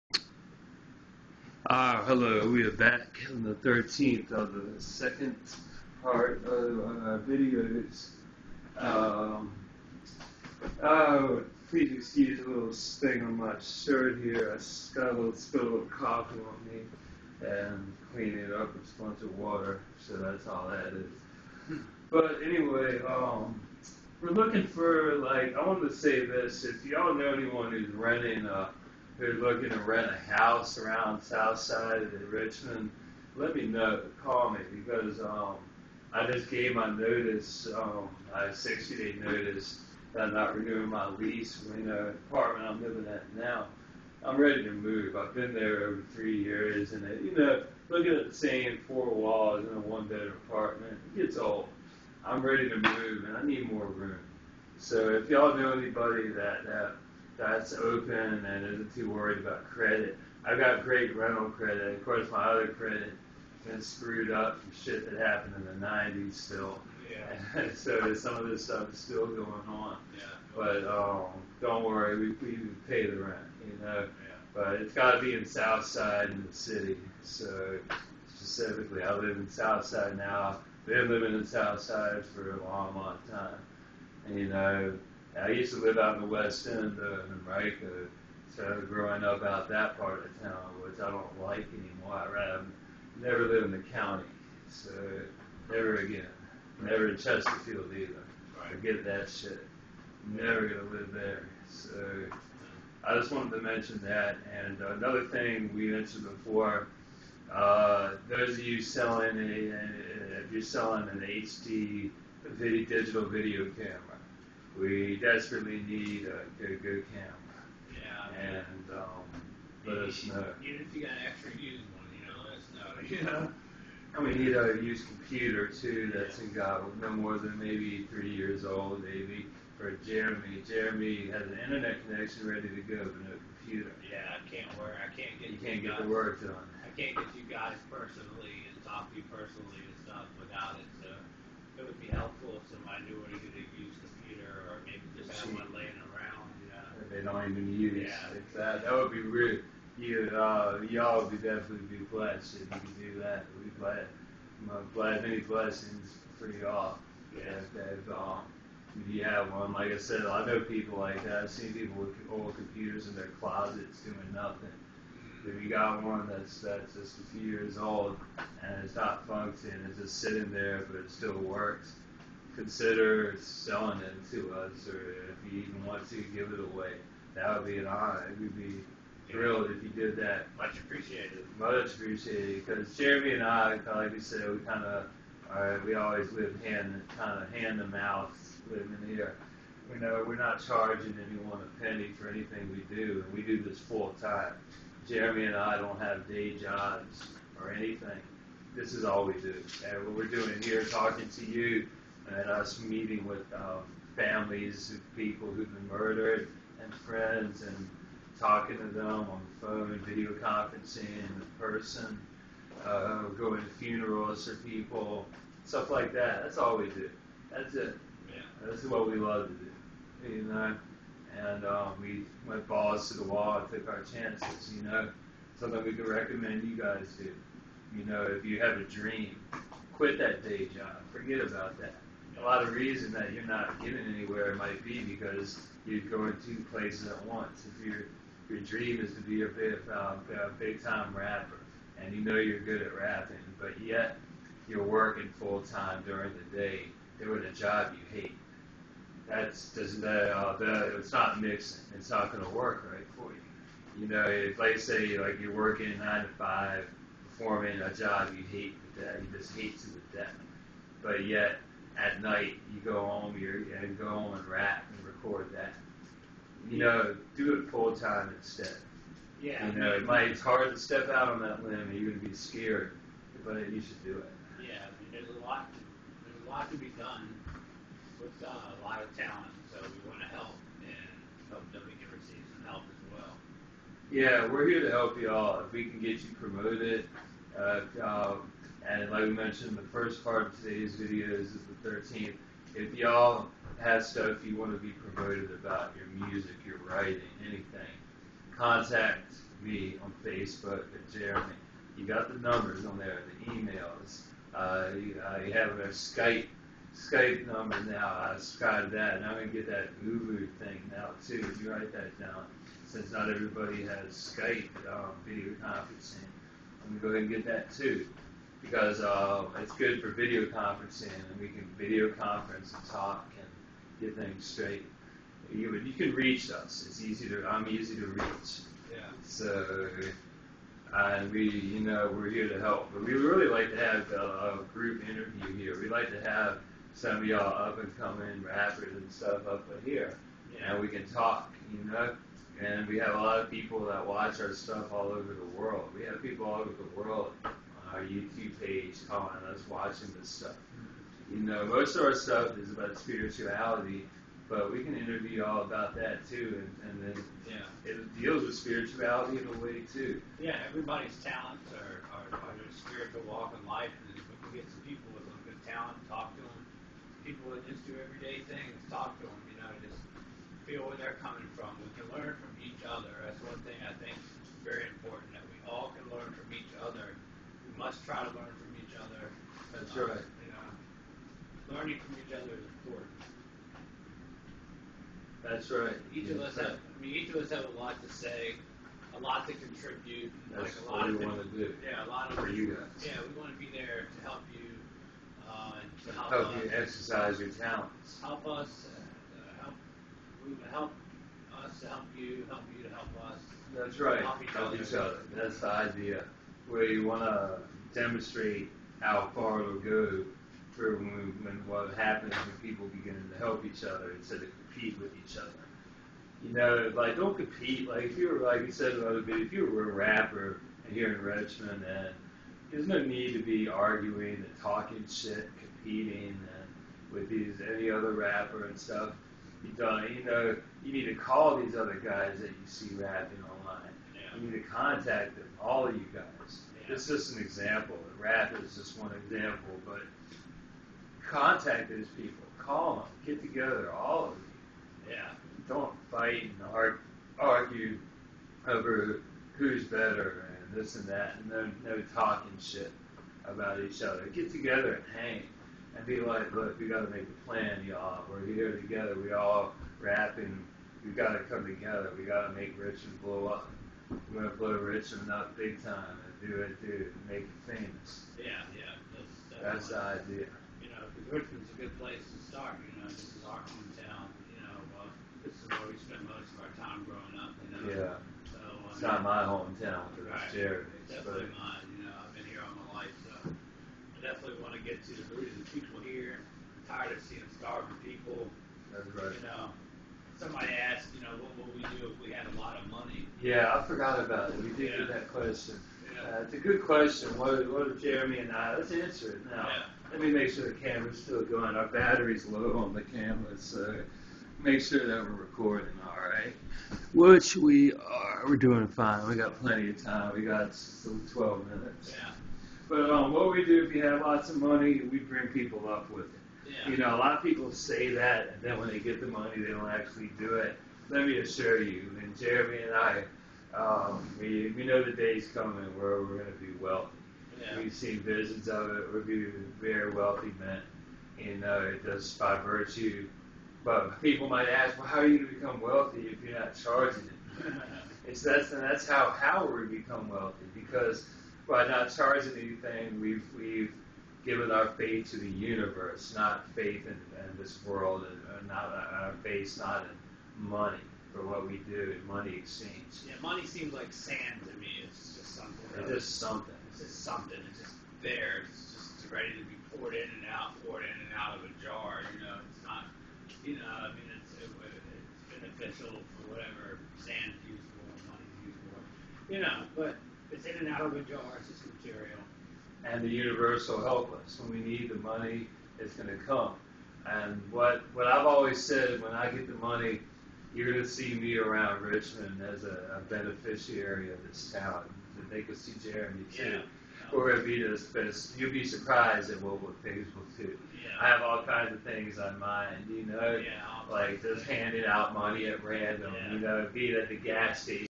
Spiritual channeling -- Lord Buddha & the Infinite Spirit...and the Beatles....part 1
We open up and talk about ways to bring people together in this world. and in our city, Richmond, VA. As were spoke, various spirits came through to offer advice. You will find this interesting.